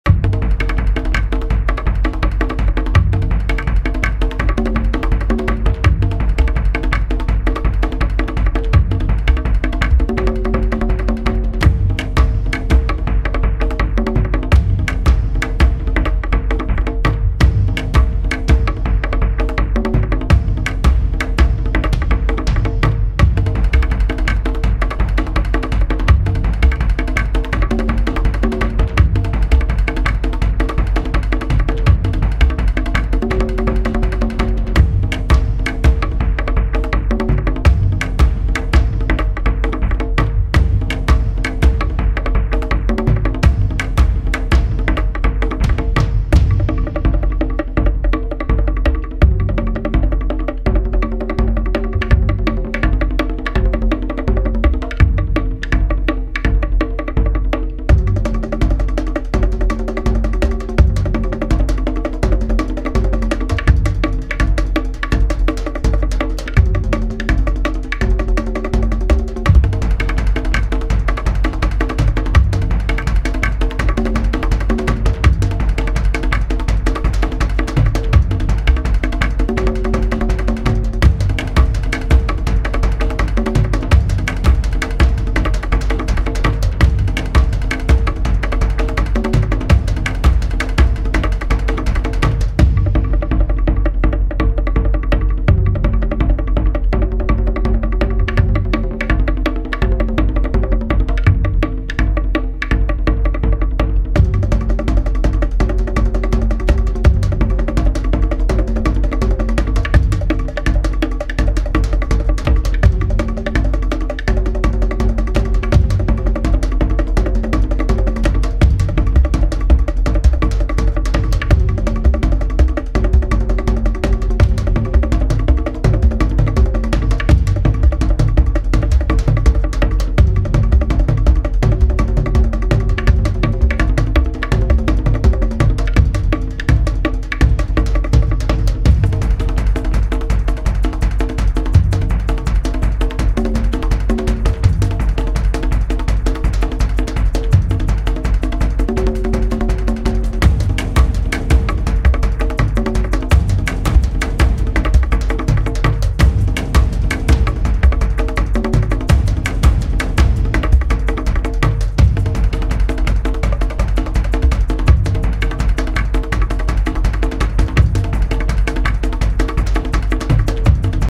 Selfie. 181 Things about Cats Your Teachers Wouldn't Tell You
I closed the bathroom door to find her like this. So that was weird.

Tags:pet cateyes catinabag cats_daily_meow chats catandlove cuteanimal siamesecat catslover catsofworld catsonig beautiful catgram cats_top100 catlife cute we_love_cats